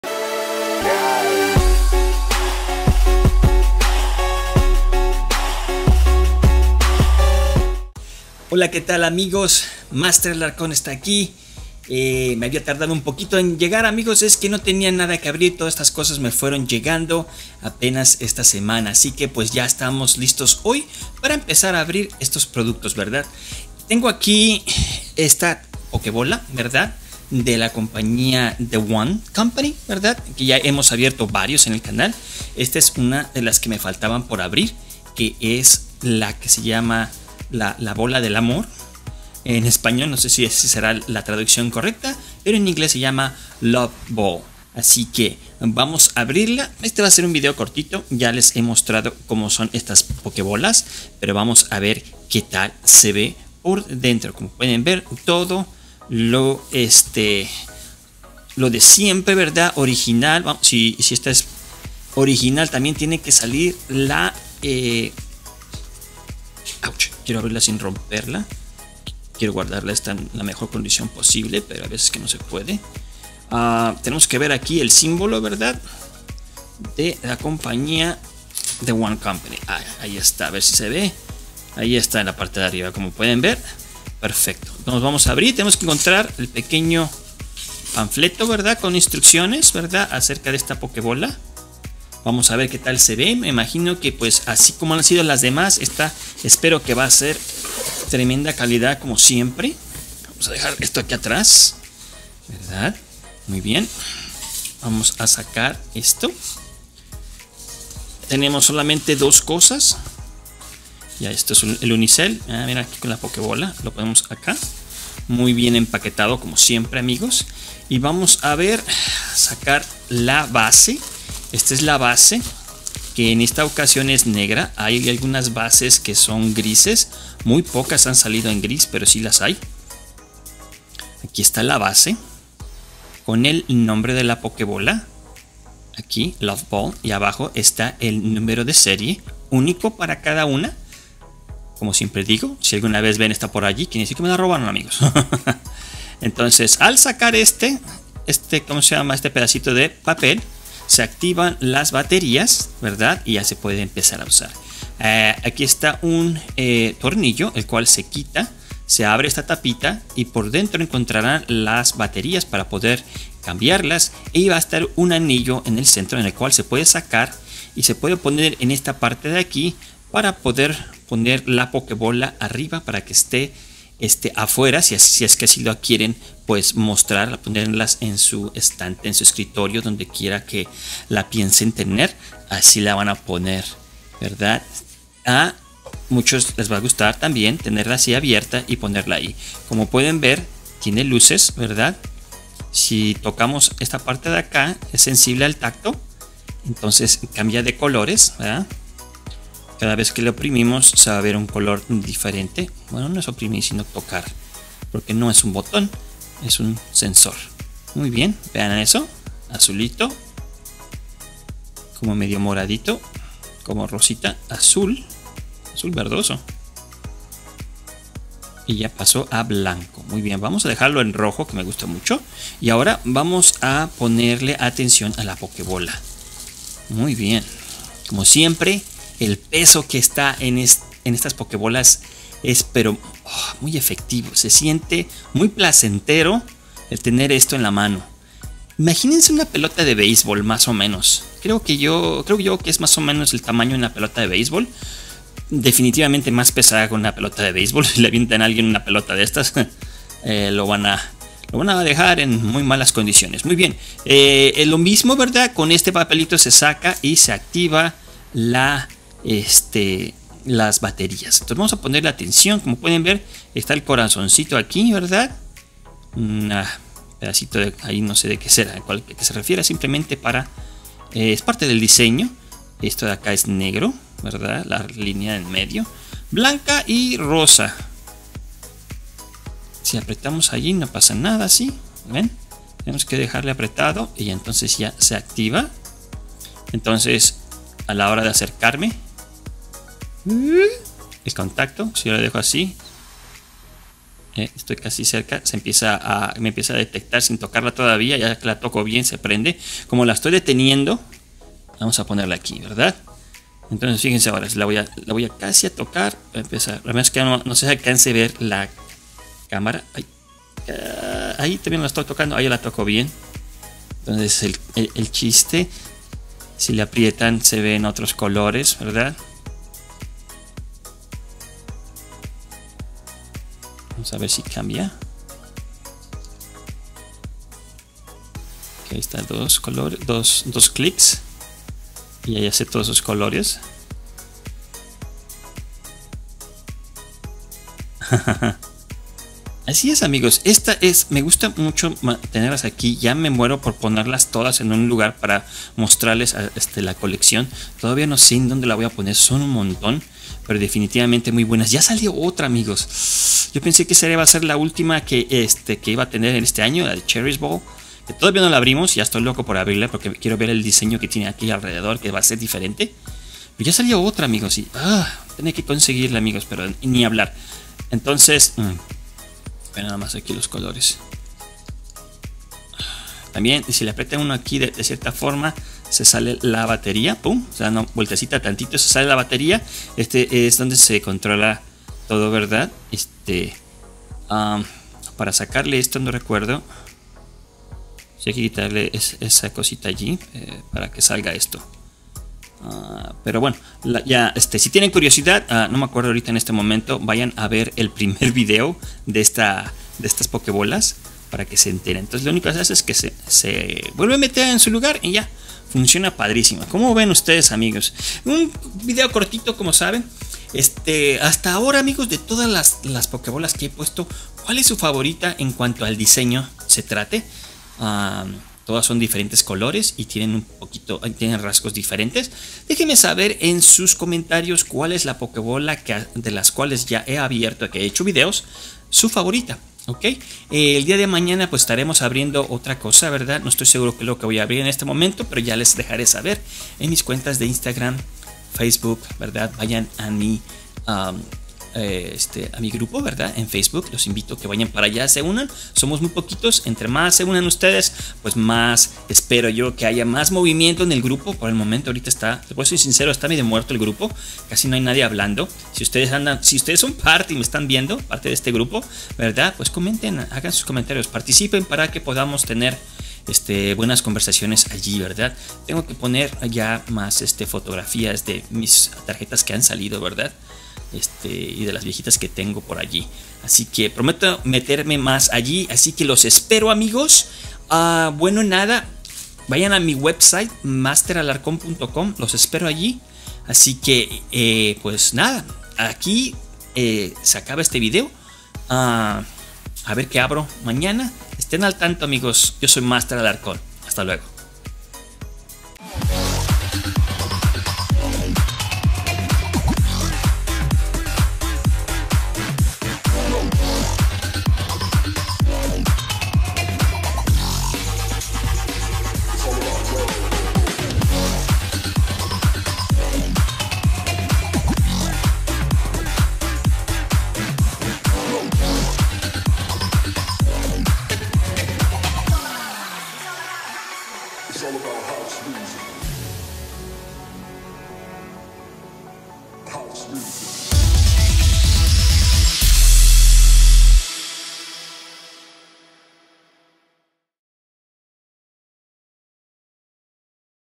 UNBOXING una Pokébola de la compañia "The wand Company" The Love Ball!
Amigos, sigo con mi colección de Pokébolas de la compañia The wand Company, esta vez es la Love Ball.

Allí podrán ver mi Blog con contenido interesante sobre el mundo de Pokémon.

Los espero!

Les doy las gracias a el Squad ya estamos en camino a los 1000 subs👍🏼
Mil gracias a ustedes mis amigos!

UNBOXING! Episodio 459